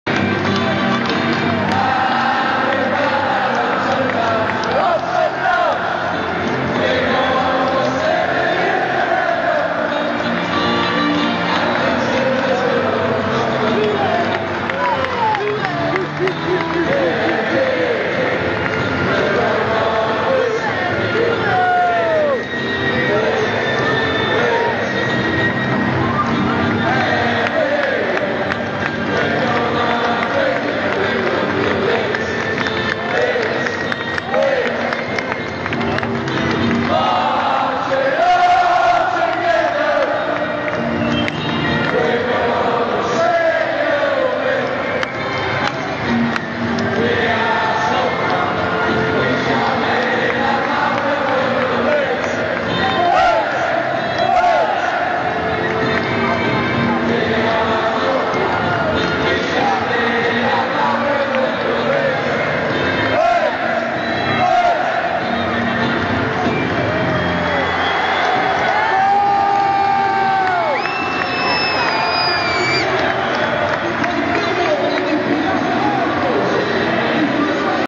leeds united end of season
leeds united finish there last game of the season and the players walk round the pitch got a great bit of rui marques as well